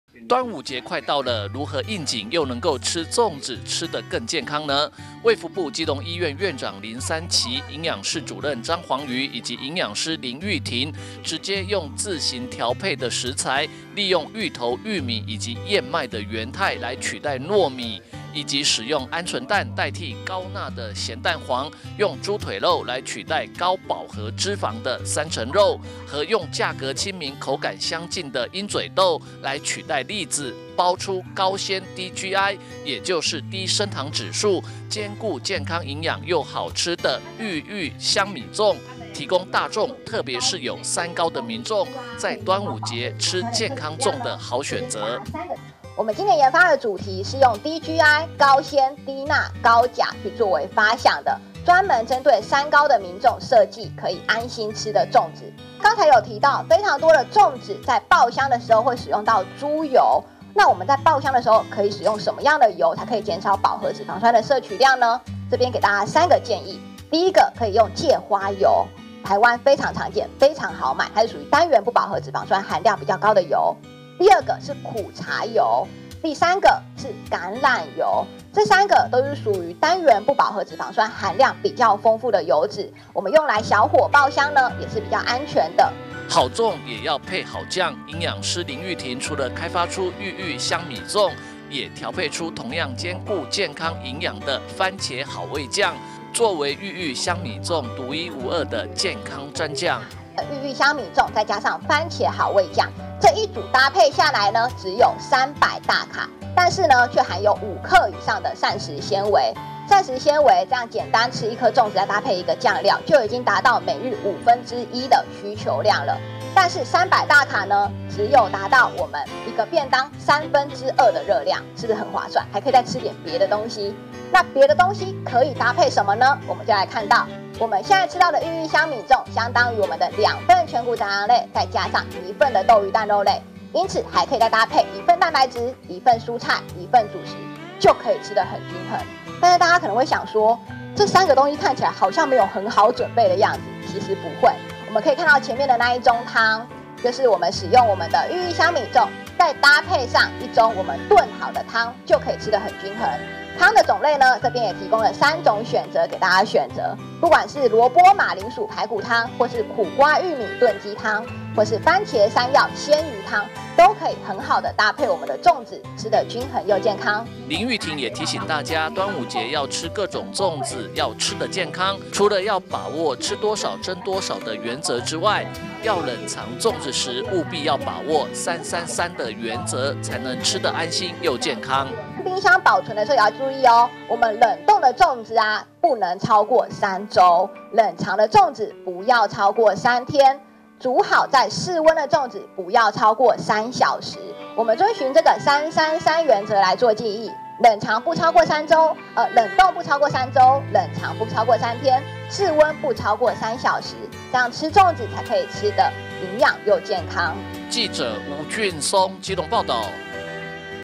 基隆|林三齊|林鈺庭|張皇瑜|衛福部基隆醫院|部基|端午節|DIY|健康粽|芋玉香米粽|蕃茄好味醬|芋頭|玉米|鵪鶉蛋|三高|高血壓|高血脂|高血糖|DIY健康粽 部基推「芋玉香米粽及蕃茄好味醬」
DIY健康粽 部基推「芋玉香米粽及蕃茄好味醬」

(中嘉吉隆新聞記者吳俊松/基隆報導)端午節快到了，要如何吃粽子吃得更健康呢？衛福部基隆醫院特地請營養師自行研發調配，製作美味又兼顧營養健康的”芋玉香米粽”以及”番茄好味醬”，也非常適合有「三高」的民眾來享用！

端午節快到了，如何應景又能吃粽子吃得更健康呢？衛福部基隆醫院院長林三齊、營養室主任張皇瑜以及營養師林鈺庭直接用自行調配的食材，利用芋頭、玉米以及燕麥的原態來取代糯米，以及使用鵪鶉蛋代替高鈉的鹹蛋黃、用豬腿肉來取代高飽和脂肪的三層肉、和用價格親民口感相近的鷹嘴豆來取代栗子，包出高纖低GI也就是低升糖指數，兼顧健康營養又好吃的「芋玉香米粽」，提供大眾，特別是有三高的民眾，在端午節吃健康粽的好選擇！

好粽也要配好醬，營養師林鈺庭除了開發出「芋玉香米粽」，也調配出同樣兼顧營健康營養的「番茄好味醬」，作為「芋玉香米粽」獨一無二的健康蘸醬！

林鈺庭也提醒大家，端午節要吃各種粽子要吃得健康，除了要把握「吃多少蒸多少」，以及要冷藏粽子時務必把握”333”的原則，才能吃得安心又健康！